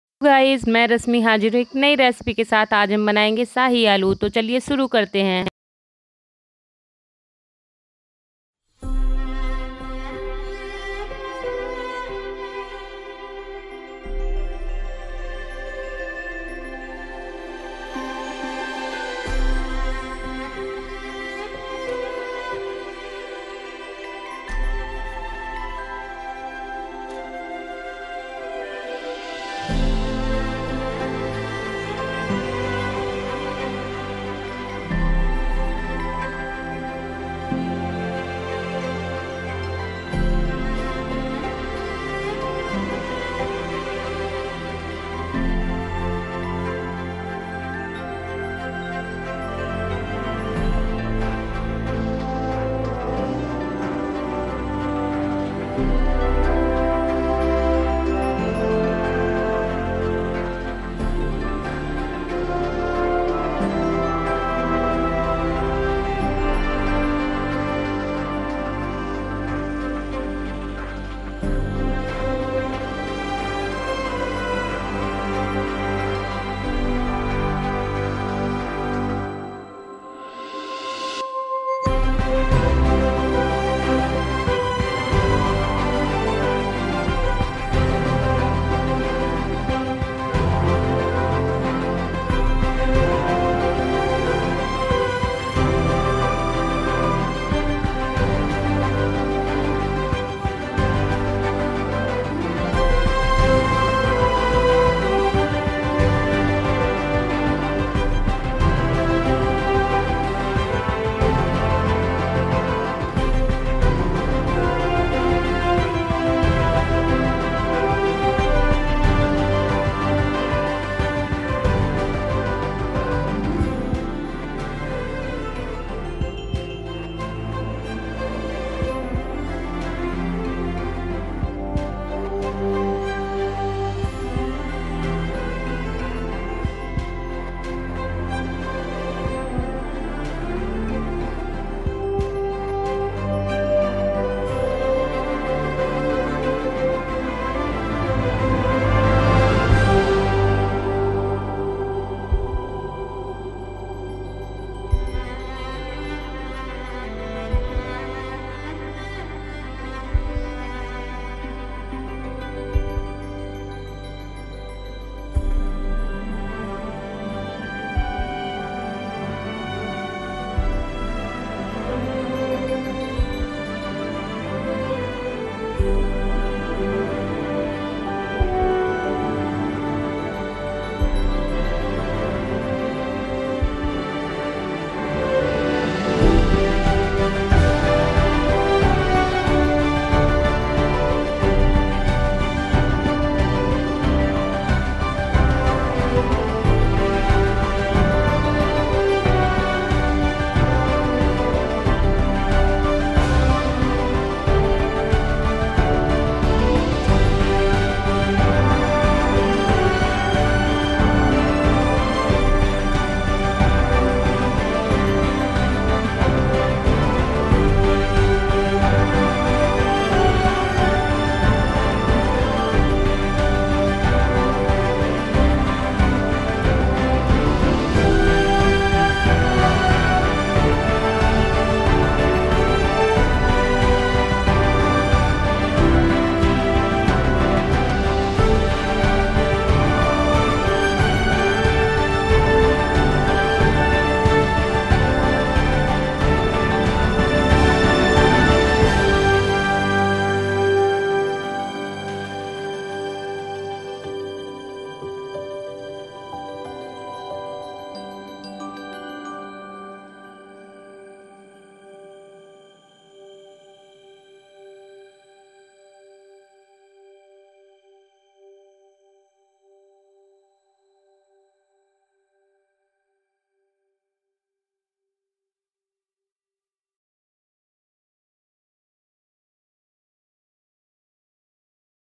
[रहस्य] शाही आलू की सब्जी बनाने का आसान तरीका | Learn the Secret of Making a Delicious Shahi Aloo!
Ingredients :-
1- Small potatoes - 15 to 20
2- Chopped Tomato - 2
3- Garlic - Full 3
4- Onion - large piece - 2
5- Chilli - 4
6- Ginger - large piece
7- Shahi masala - 1 and a half teaspoon
8- Garam masala - 1 teaspoon
9- Turmeric powder - half teaspoon
10- Coriander powder - 2 teaspoon
11- Salt - to taste
12- Oil - as required
13- water- as required
14- Dry Fenugreek leaves

Equipment :- Sumsung phone hi video banati hu.

Aloo recipe  ,Aloo ki rasedar sabji